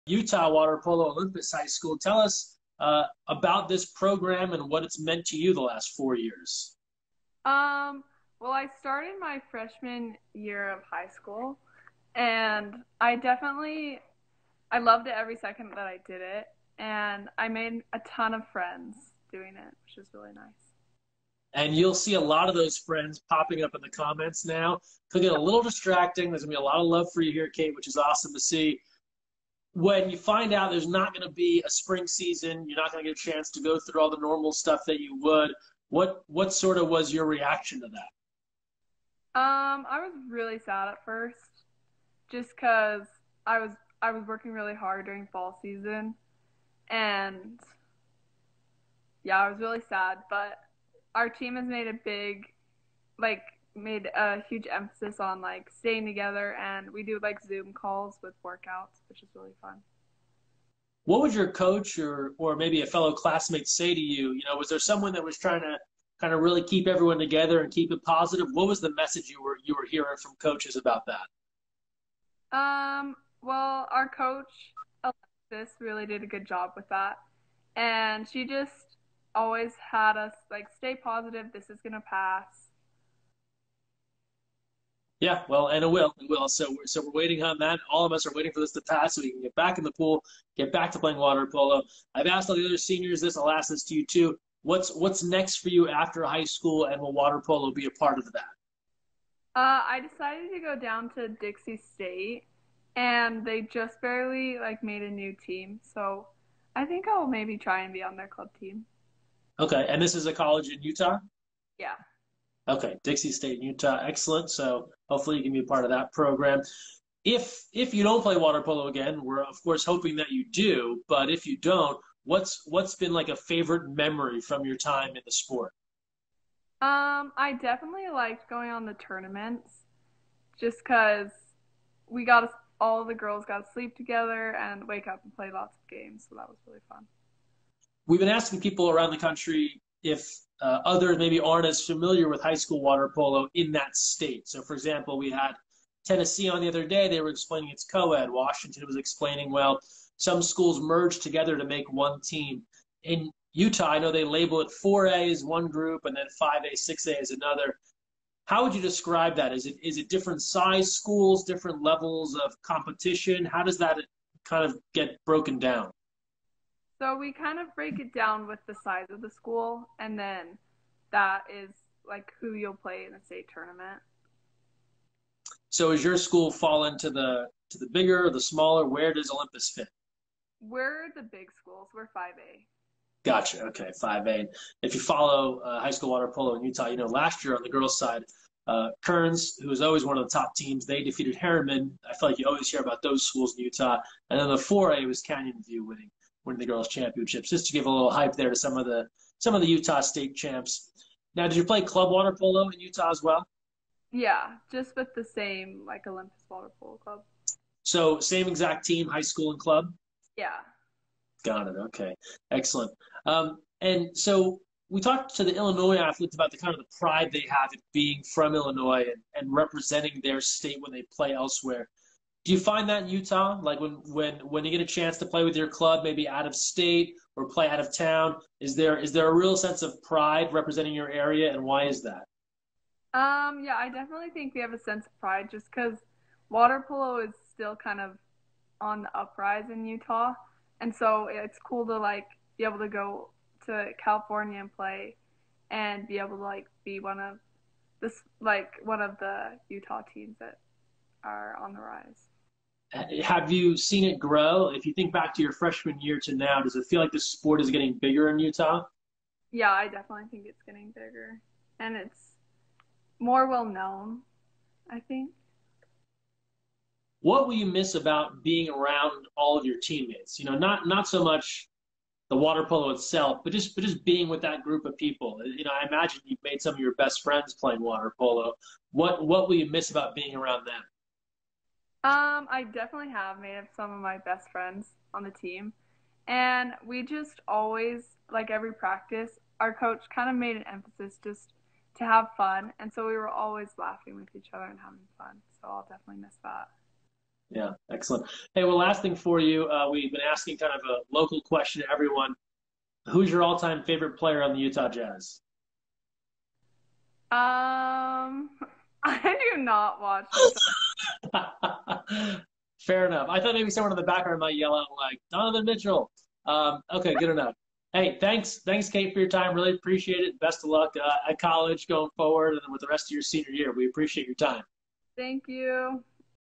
2020 Instagram Interview: Kate Gregory
Olympus High School (Holladay, UT) Senior, Kate Gregory reflects on her time on the Varsity WaterPolo team, looks ahead to the future and chats waterpolo growth with Greg Mescall!